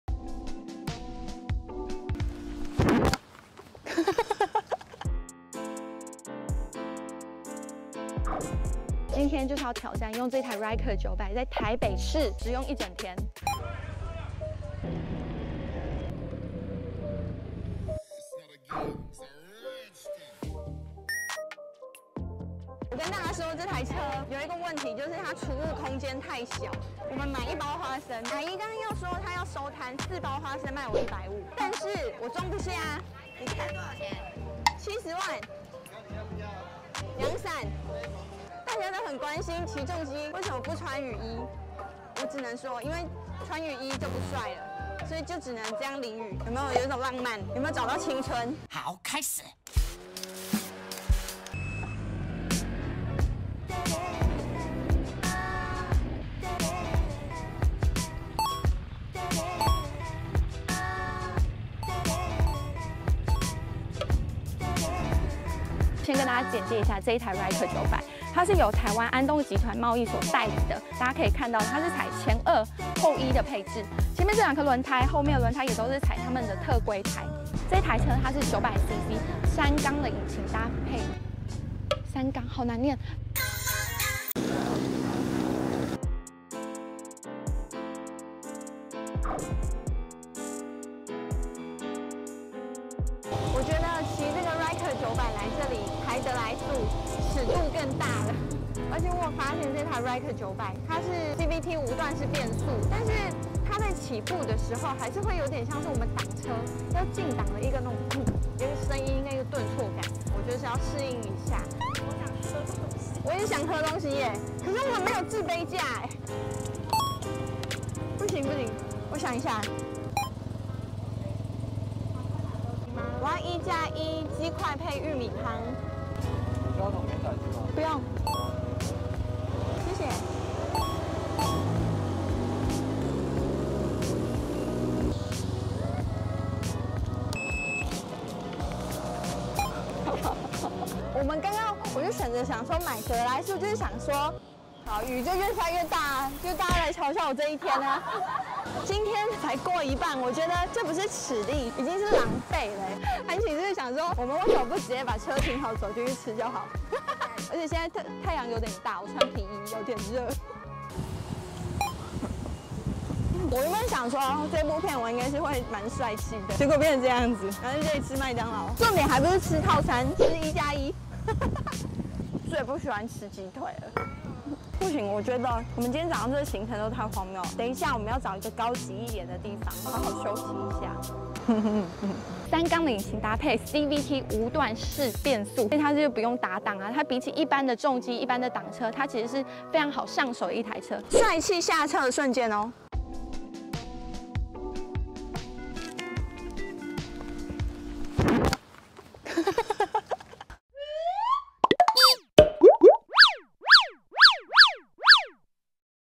【皮衣正妹駕駛三輪重機超吸睛！ 瀟灑下車霸氣外露帥翻網：戀愛了】｜@haileyhsu
騎重機的女生真的很酷欸！(BK編)
創作授權【徐海莉 Hailey】